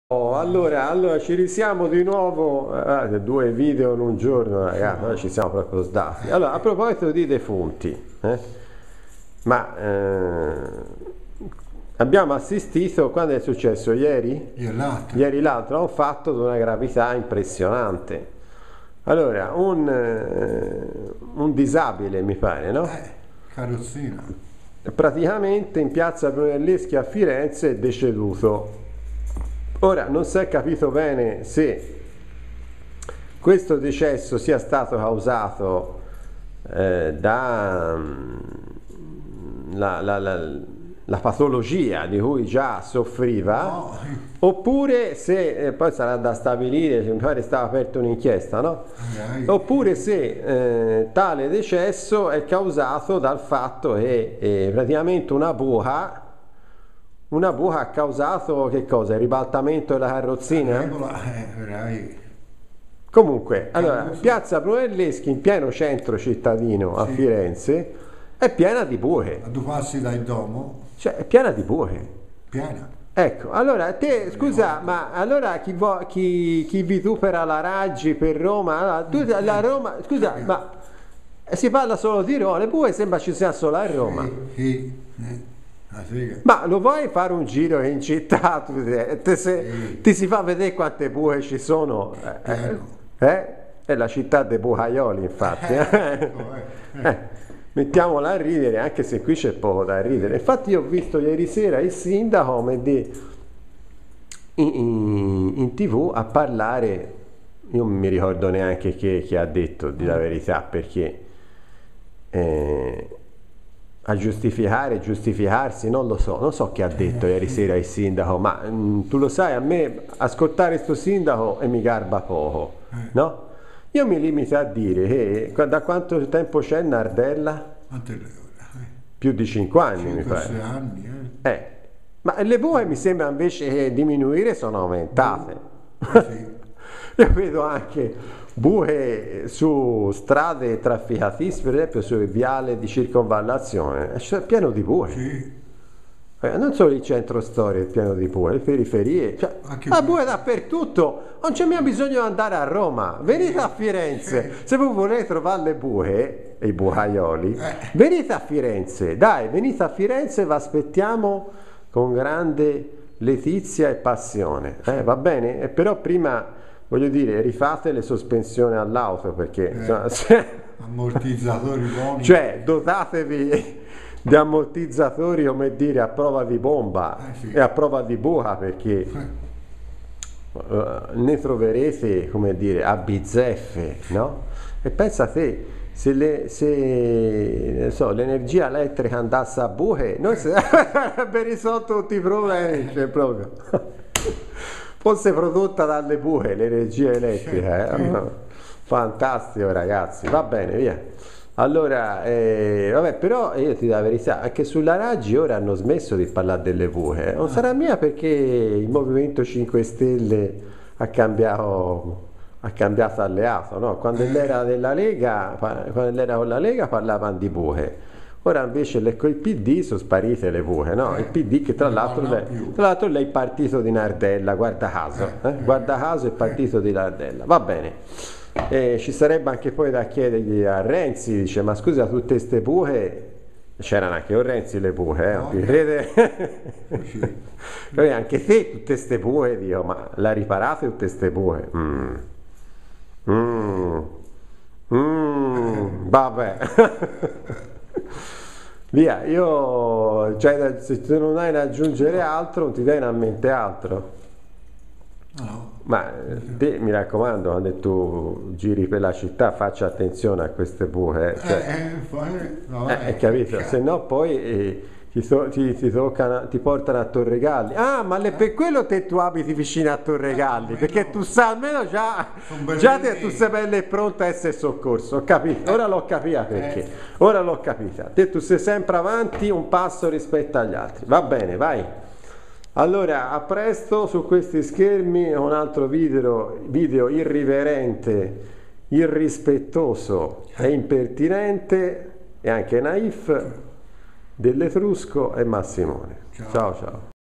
MORTE SOSPETTA di un disabile: Firenze sprofonda sempre più in BUCA. SARA' MICA COLPA DELLA RAGGI..?
E meno male che il Sindaco di Firenze NON si chiama Virginia Raggi, se no l'avrebbero FATTA A FETTE e con quelle RIEMPITO LE BUCHE...!